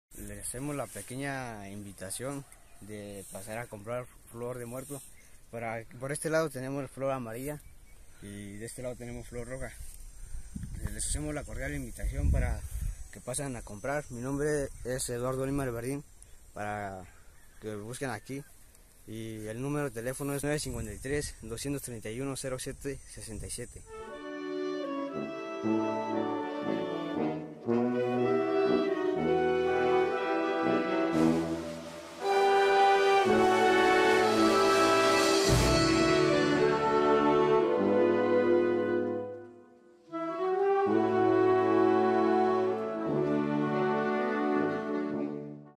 Familias de Santiago Huajolotitlán se apoyan de la venta de flores para la temporada de muertos
Familias de Santiago Huajolotitlán se apoyan de la venta de flores para la temporada de muertos, los invitan a pasar por sus flores a campo.